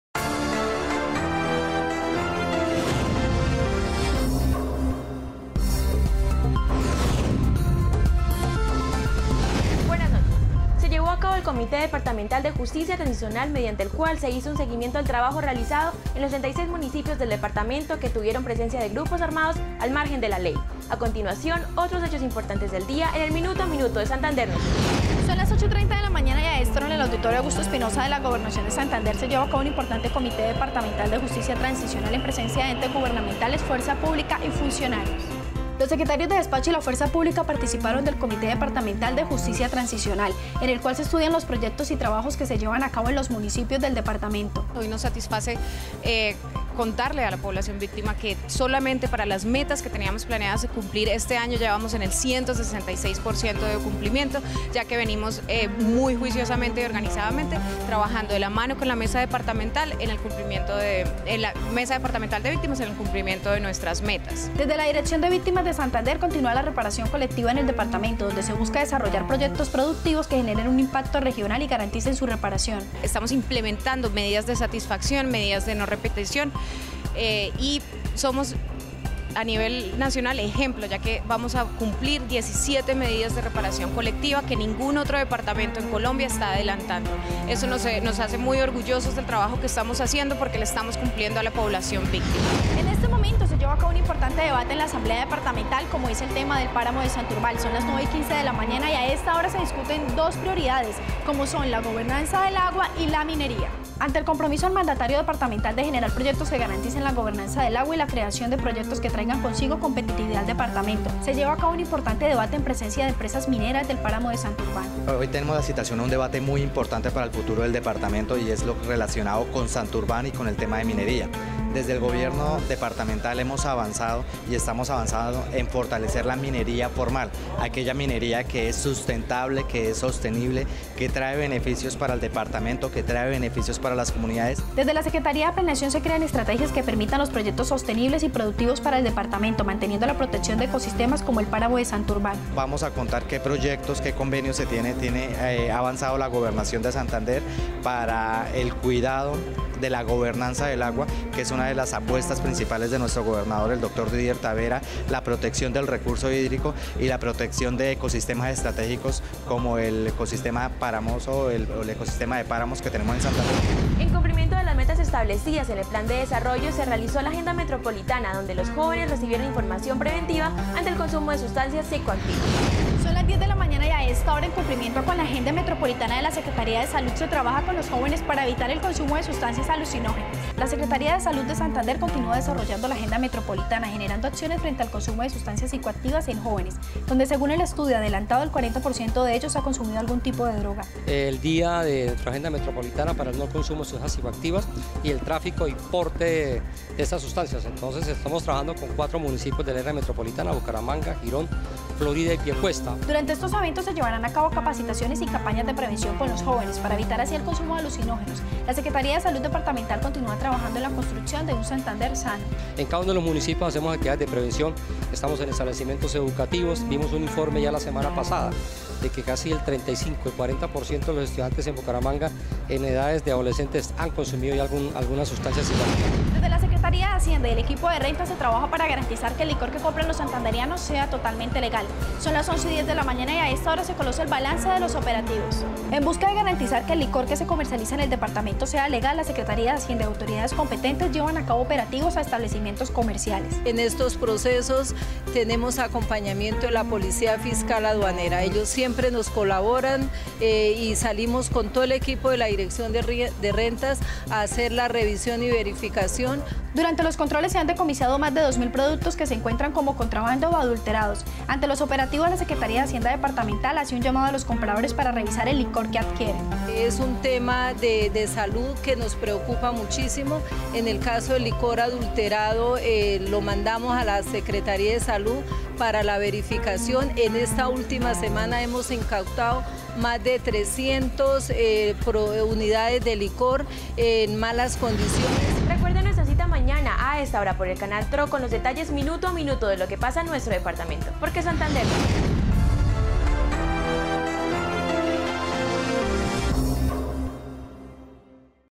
Minuto a minuto 28 de junio
¡Adelante por un Santander Competitivo! Conozcan los hechos más importantes que se desarrollaron el 28 de junio en la Administración Departamental.